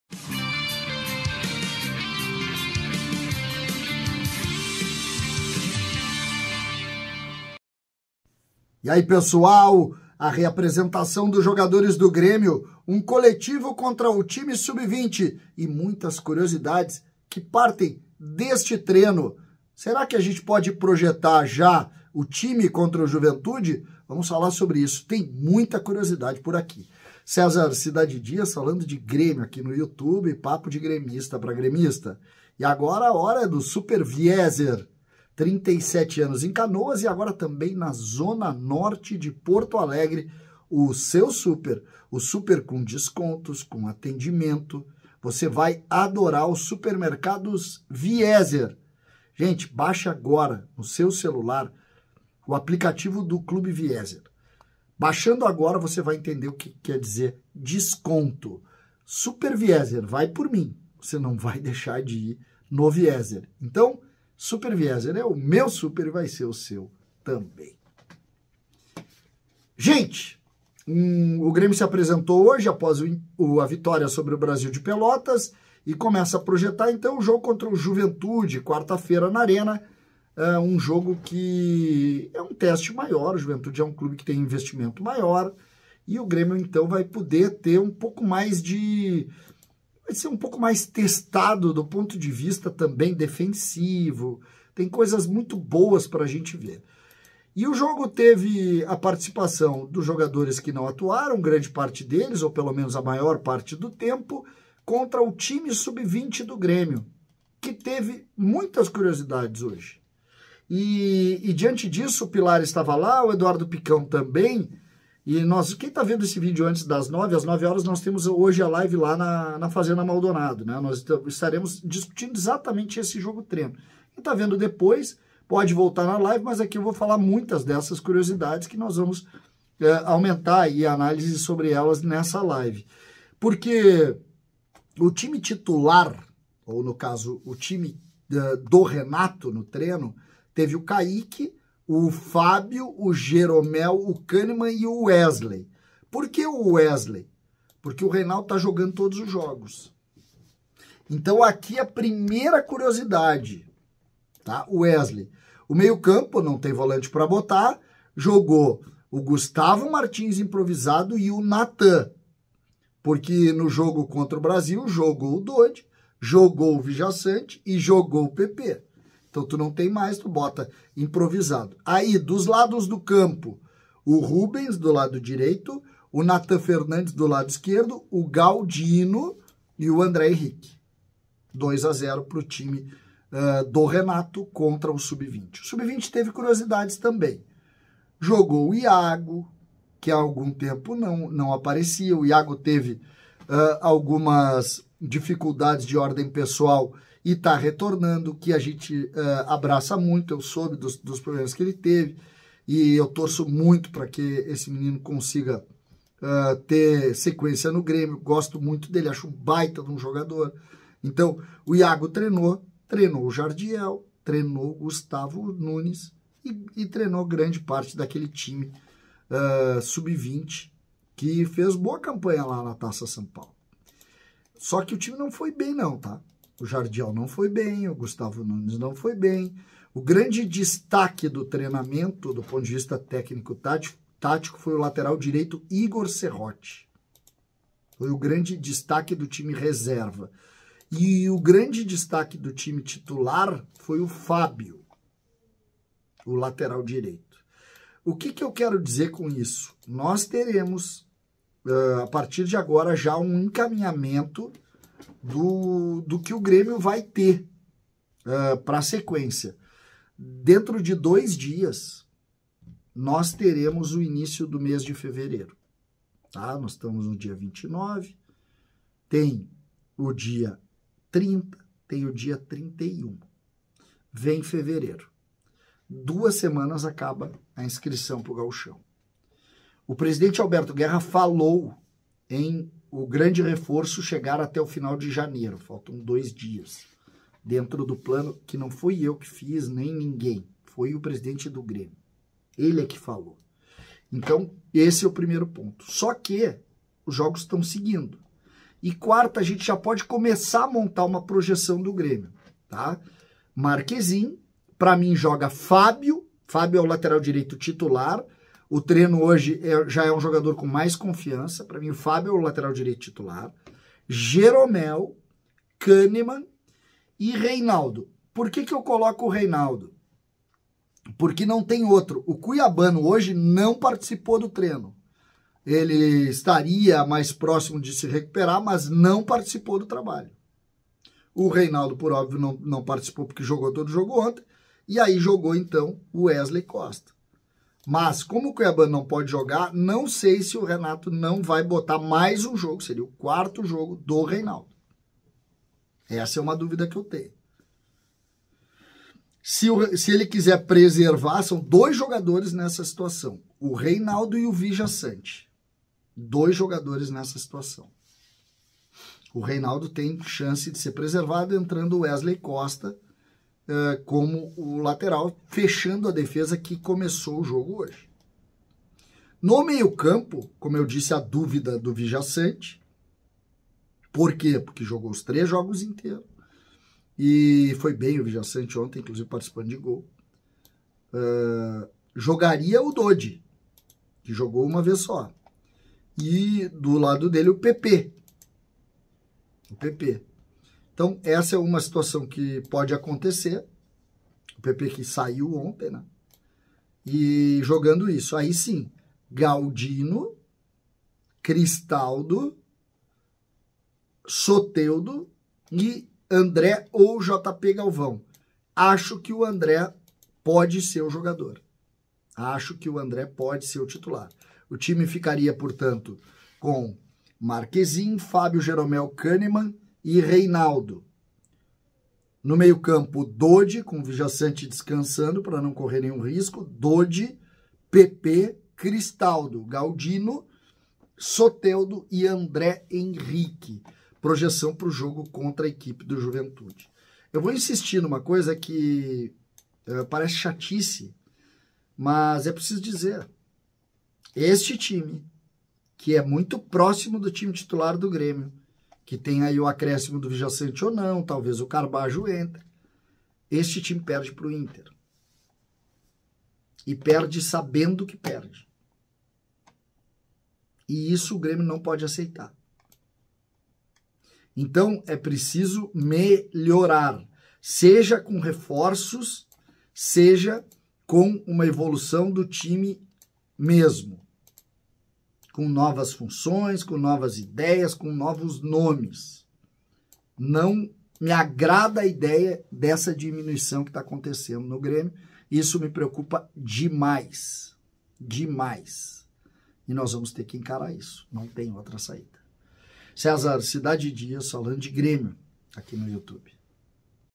Geromel, Gustavo Nunes, Fábio, time de quarta-feira... as curiosidades do treino do Grêmio
SUPERMERCADOS VIEZZER